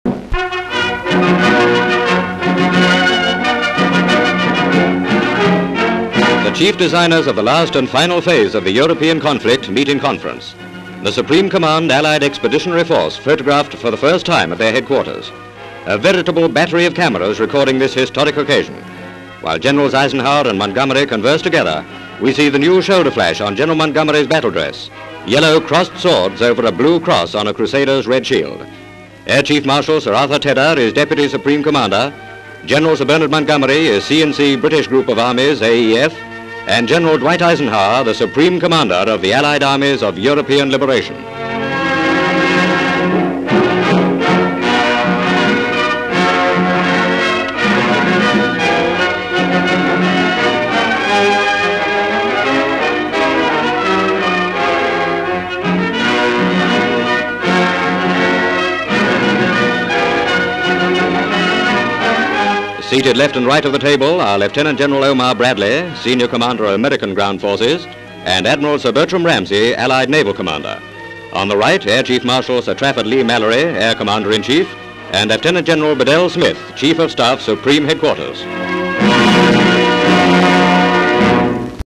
Allied Chiefs Meet In London (1944)
Item title reads - Allied Chiefs meet in London.

First ever pictures of the Allied Chiefs at their London Headquarters. They study a map on the wall and then pose for photographers. Seen are General Dwight Eisenhower, General Sir Bernard Montgomery and Air Chief Marshal Sir Arthur Tedder. They sit around a table, also present are Lieutenant General Omar Bradley, Admiral Sir Bertram Ramsey, Air Chief Marshal Sir Trafford Leigh-Mallory and Lieutenant General Biddell-Smith.
 FILM ID:1350.18